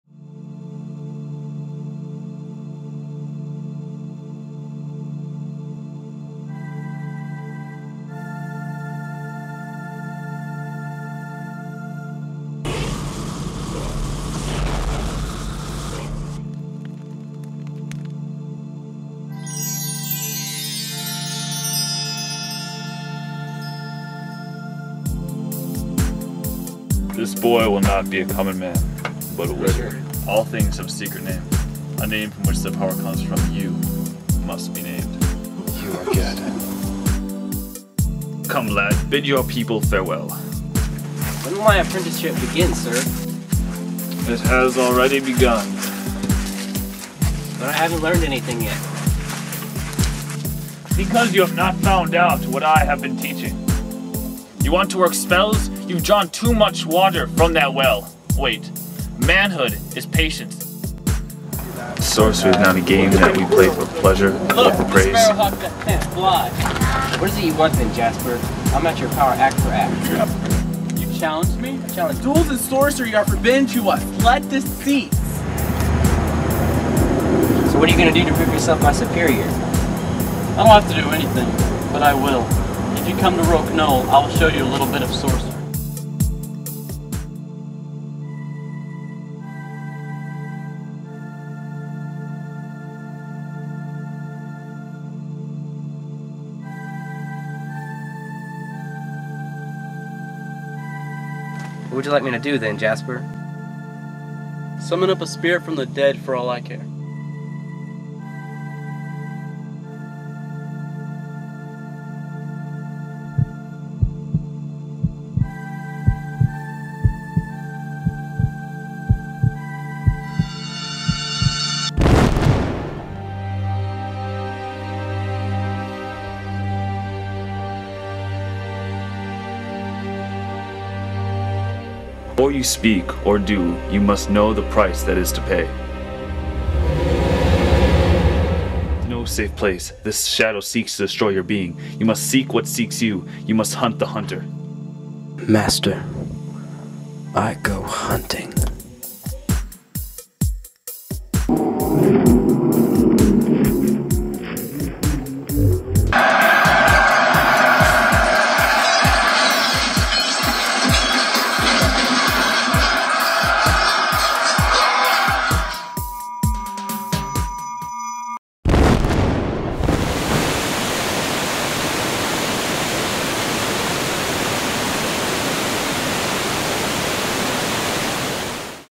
A Wizard of Earthsea Book Trailer
Booktrailer I did my Junior year at high school

Its funny. well maybe if you were there you would find it funny